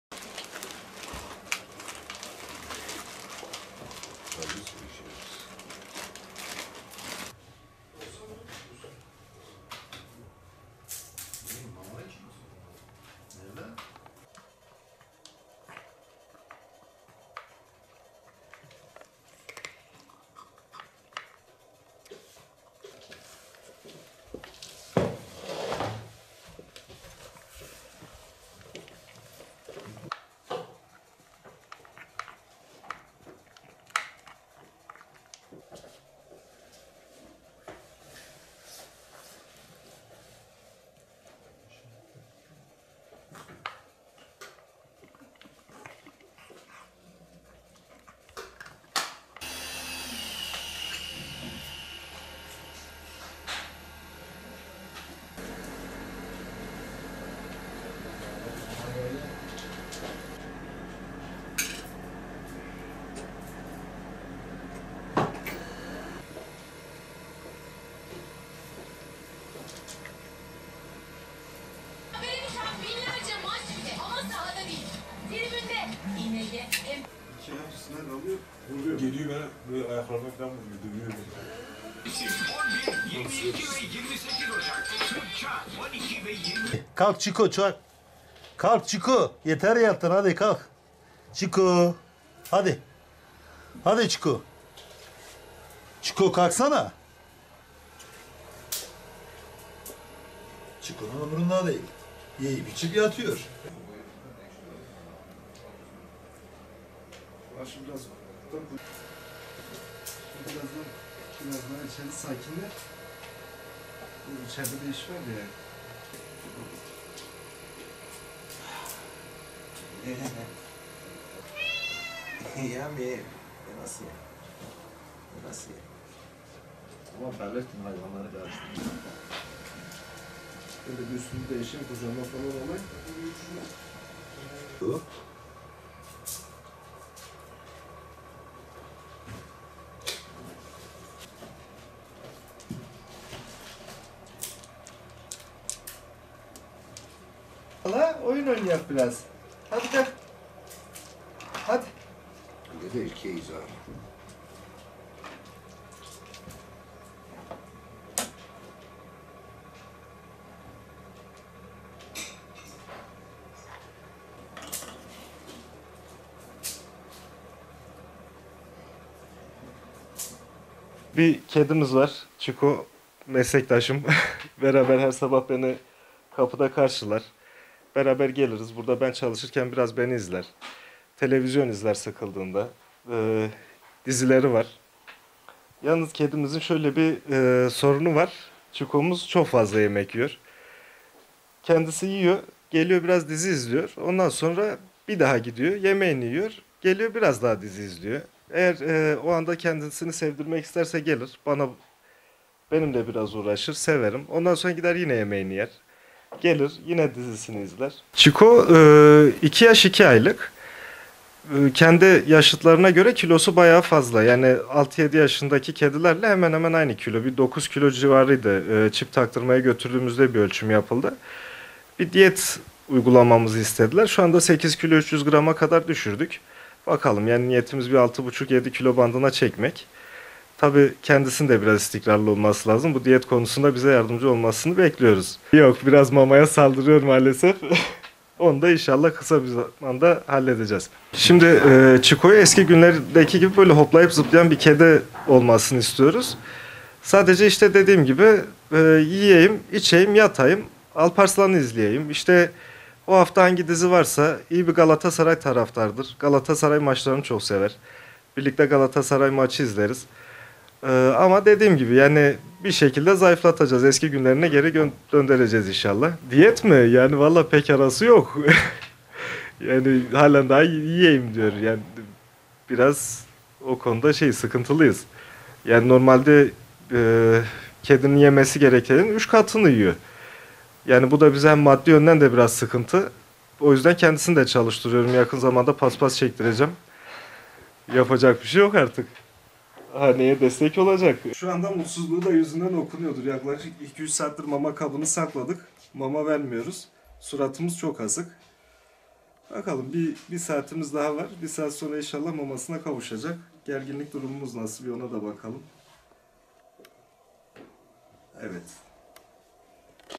9 kiloluk 'Çiko' zayıflamak için diyete başladı
Elazığ’da cinslerinden 2 kat daha büyük olan 'Çiko' adlı kedi görenleri şaşırtıyor. Kedi normalin 3 katını yiyip 9 kiloya ulaşınca, sahibi mamasına yetişemedi ve kedisini diyete başlattı.
Elazığ’da yaşayan Türk Tesbih Sanatı Tesbih Sanatçısı Turgut Uğur Duman’ın 'Çiko' adlı British Shorthair cinsi kedisi 9 kilo ağırlığıyla dikkatleri üzerine topluyor. 2 yaşındaki Çiko'un hobisi, yemek yemek ve uzanıp televizyon izlemek. Duman, kendi cinslerinden kat kat daha iri durumda olan kedisinin mamasını yetişemeyince diyet uygulamaya başladı. Şimdiye kadar 700 gram zayıflayan Çiko’nun diyetle arasının iyi olmadığı öğrenildi.

"Diyetle pek arası yok hala yiyeyim diyor"
Çiko’nun kendi yaşıtlarına göre kilosunun fazla olduğu ve bundan dolayı diyet uyguladıklarını dile getiren sahibi Turgut Uğur Duman," Çiko adından bir kedimiz var. Beni her sabah kapıda karşılar. Burada ben çalışırken beni izler, sıkıldığında televizyonda dizileri var onları izler. Yalnız kedimizin şöyle bir sorunu var. Çiko çok fazla yemek yiyor. Kendisi yiyor, geliyor biraz dizi izliyor. Ondan sonra bir daha gidiyor yemeğini yiyor. Geliyor biraz daha dizi izliyor. Eğer o anda kendisini sevdirmek isterse gelir benimle biraz uğraşır severim ondan sonra gider yine yemeğini yer" dedi.

"Maddi yönden de biraz sıkıntı oluyor"
Çiko'nun 2 yaşında olduğunu aktaran Duman, "Kendi yaşıtlarına göre kilosu baya fazla. Yani 6-7 yaşındaki kedilerle hemen hemen aynı kiloda. 9 kilo civarındaydı. Cip taktırmaya gittiğimizde ölçmüştük. Diyet uygulamamızı istediler. Şuanda 8 kilogram 300 grama kadar düşürdük. Niyetimiz 6 buçuk -7 kilo bandına çekmek. Tabi kendisinin de biraz istikrarlı olması ve diyet konusunda bize yardımcı olmasını bekliyoruz. Mamaya saldırıyor, onu da en yakın zamanda halledeceğiz. Çiko’yu eski günlerdeki gibi hoplayıp, zıplayan bir kedi olmasını istiyoruz. Sadece dediğim gibi yiyeyim, içeyim, yatayım Alparslan’ı izleyeyim, o hafta hangi dizi varsa izler. İyi bir Galatasaray taraftarıdır. Galatasaray maçlarını çok sever. Birlikte Galatasaray maçı izleriz. Ama dediğim gibi bir şekilde zayıflatacağız, inşallah yine eski günlerine geri döndüreceğiz. Normal bir kedinin yemesi gerekenin 3 katını yiyor. Bu bize hem maddi yönden de biraz sıkıntı oluyor o yüzden kendisini de çalıştırıyorum. Yakın zamanda paspas çektireceğim yapacak bir şey yok, oda haneye destek olacak" diye konuştu.